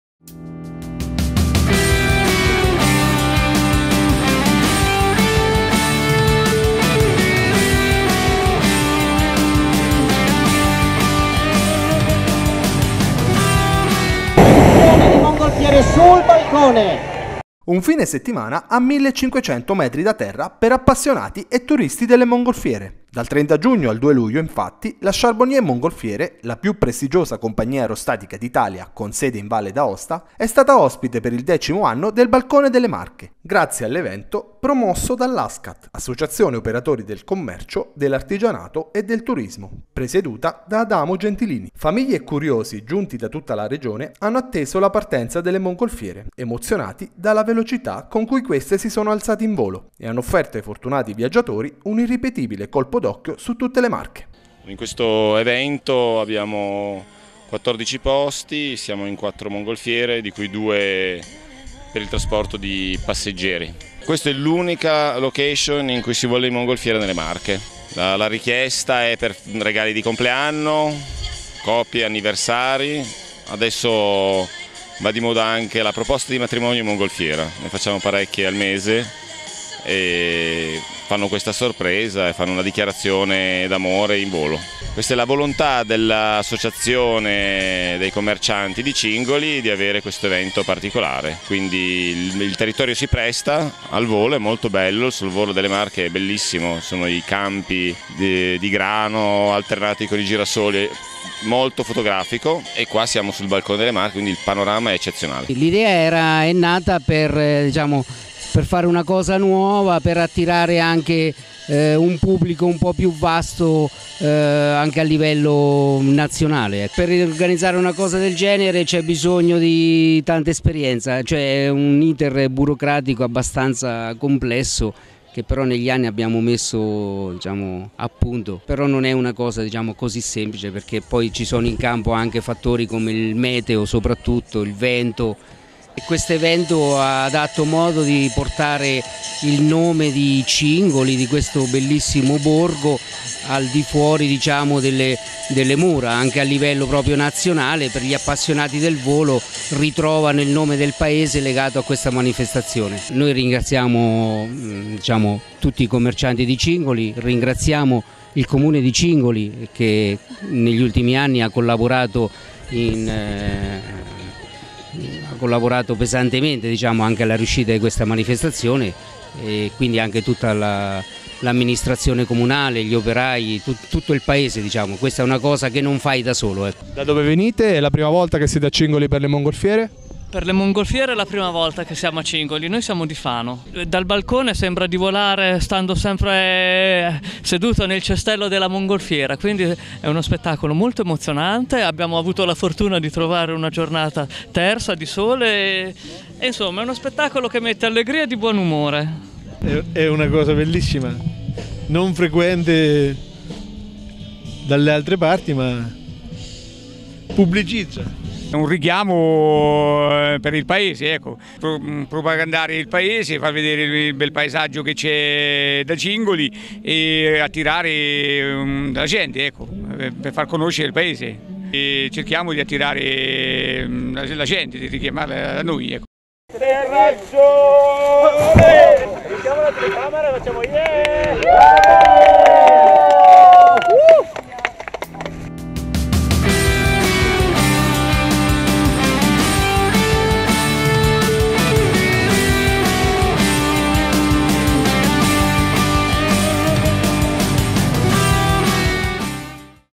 NEWS. Le mongolfiere sul cielo di Cingoli
Emozionante spettacolo dal Balcone delle Marche: quattro mongolfiere si sono alzate in volo da Cingoli per un colpo d'occhio d'eccezione su tutto il territorio regionale. Vediamo...